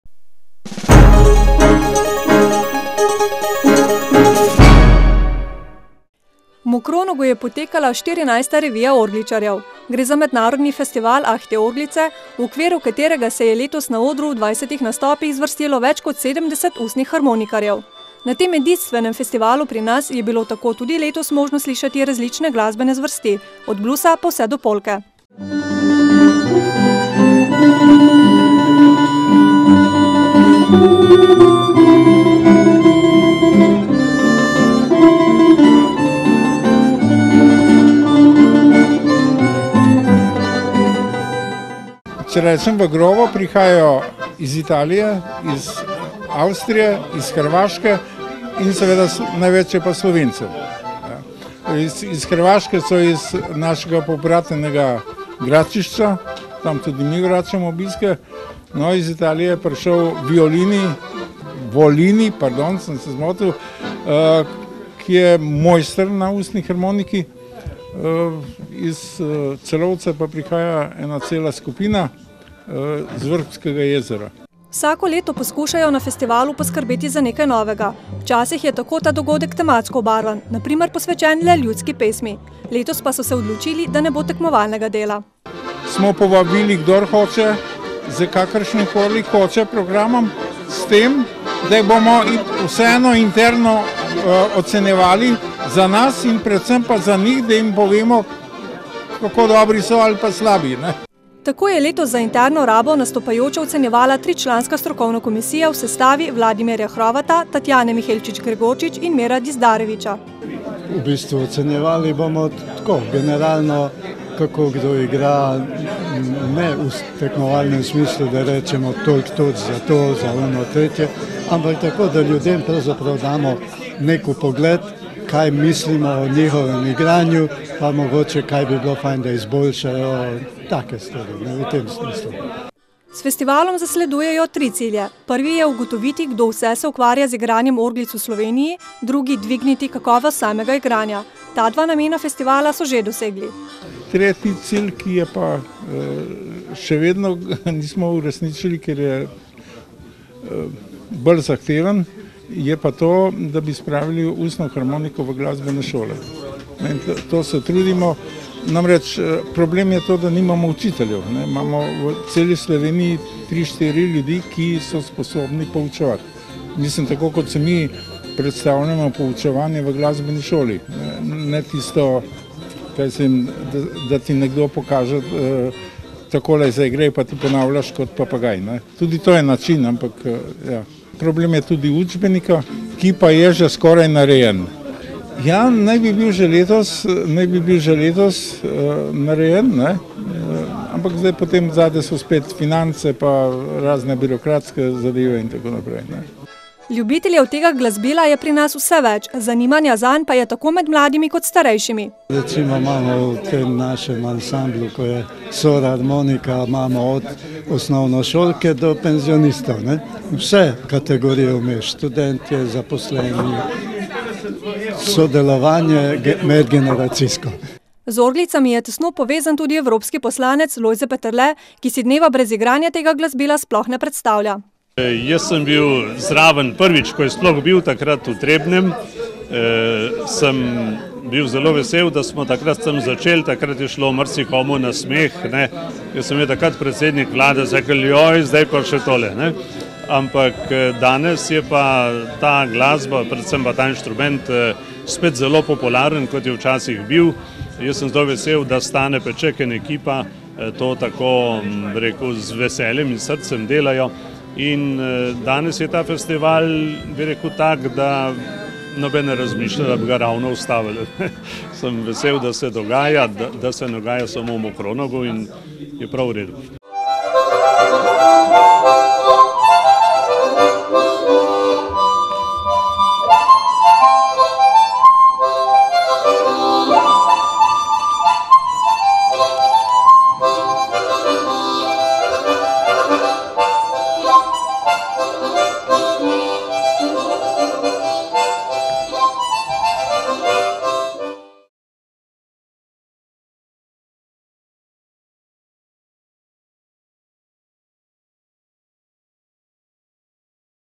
Kultura: Ah, te orglice
MOKRONOG - V Mokronogu je potekala 14. revija orgličarjev - gre za mednarodni festival "(ah), TE ORGLICE", v okviru katerega se je letos na odru v 20 nastopih zvrstilo več kot 70 ustnih harmonikarjev.